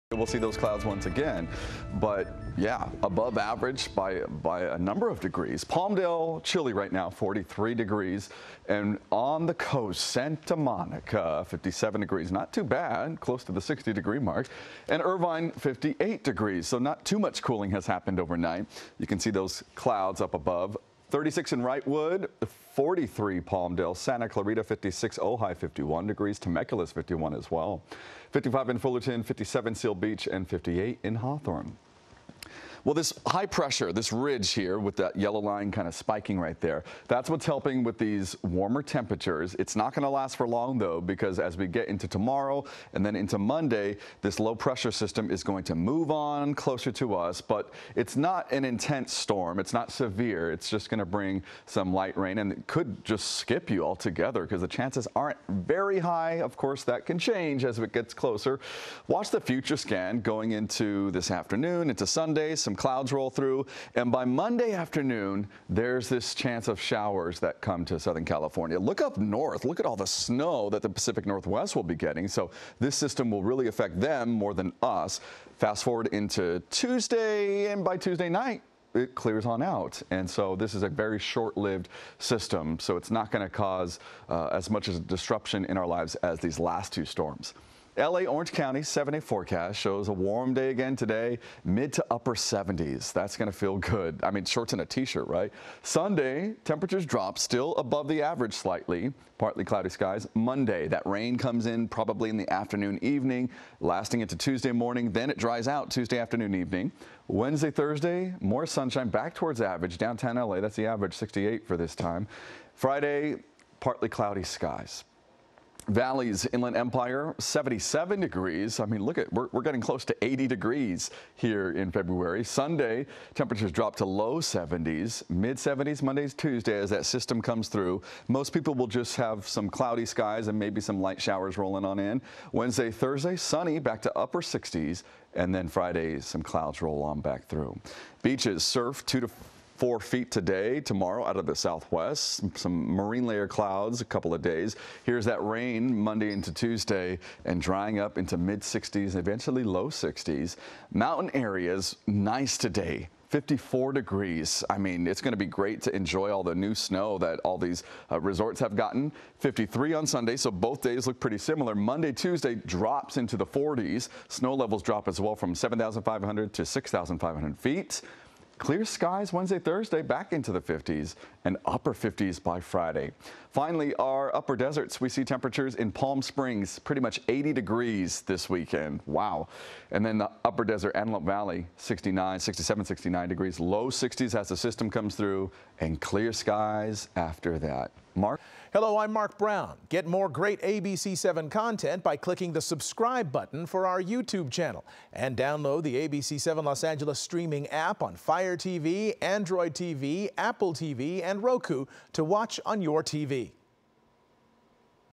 Clear, mild weekend ahead in SoCal but more rain will start off the week
Southern California can enjoy a few days of clear skies and pleasant temperatures before a new round of rain moves in.